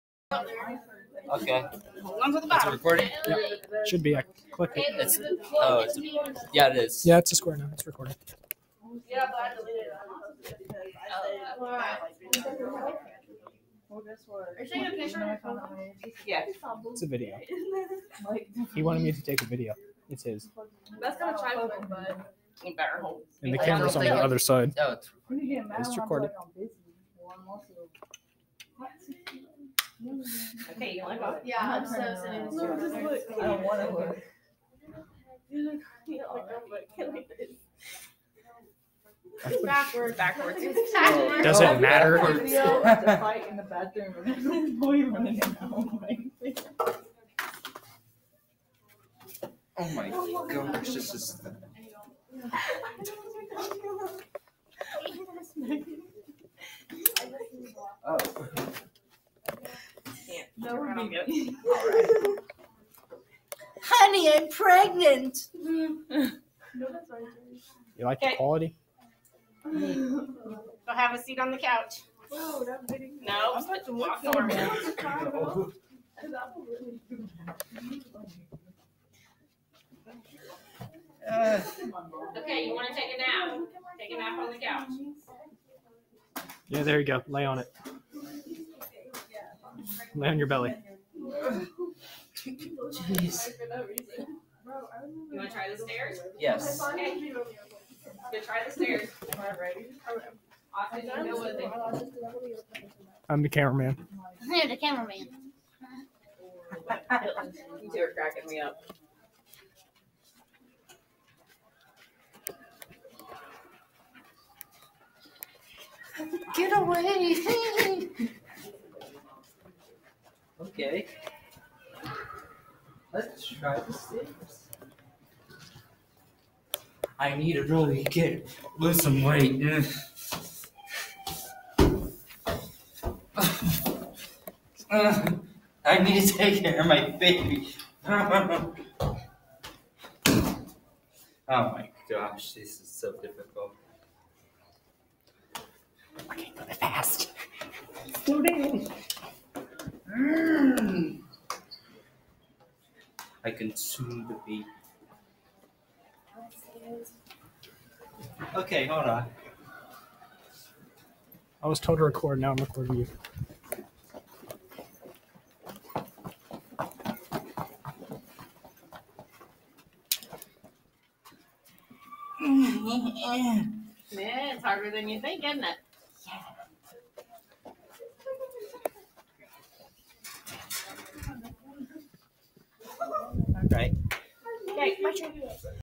Becoming Pregnant in Child Development (CRINGE)
I simulate pregnancy in my 6th period.